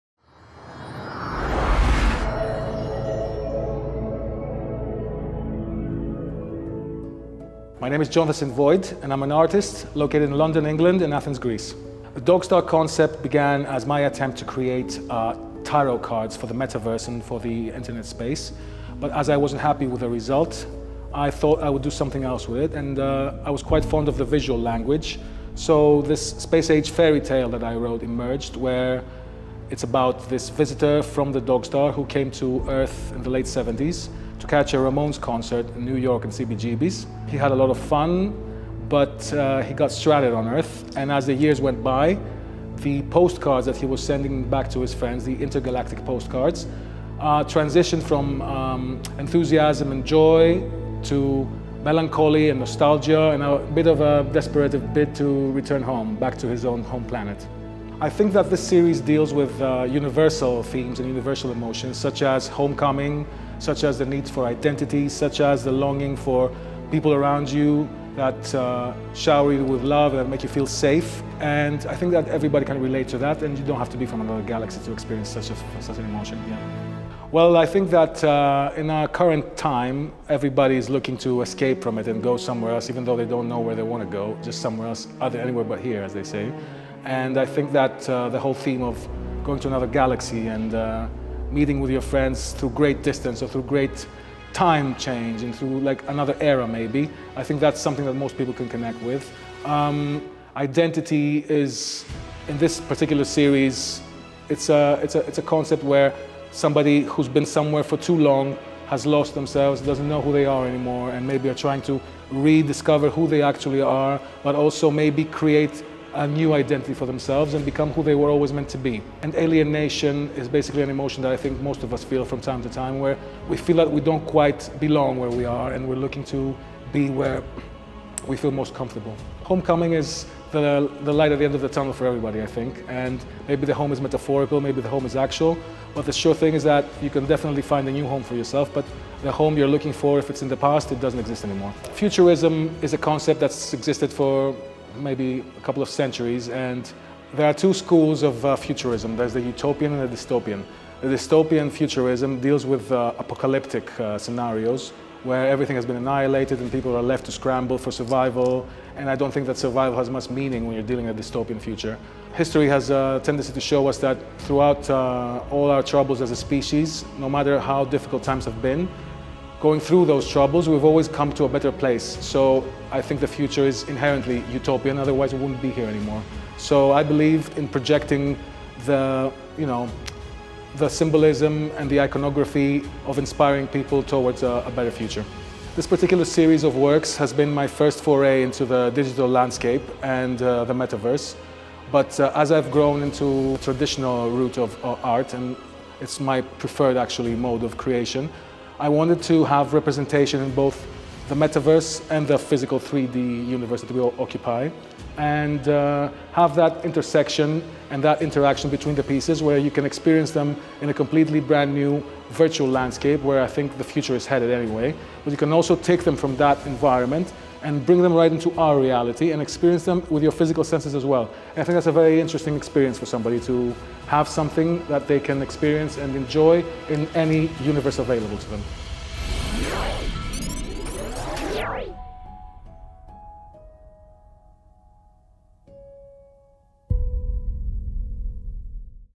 The Blender Gallery presents: Jonathan St. Void | Full Interview
In the context of the group show "Above Olympus to the Dogstar", which took place at The Blender Gallery in December 2023, Jonathan St. Void gives an interview about his latest series of the series "They Came From The Dogstar". St. Void's ‘Intergalactic Postcards’ in the form of canvas paintings, custom animated light-boxes, Virtual Reality environments, digital video projections and NFTs are a combination of the magic and wonder of Science Fiction with the seductive danger and the rebellious spirit of Rock n’ Roll culture.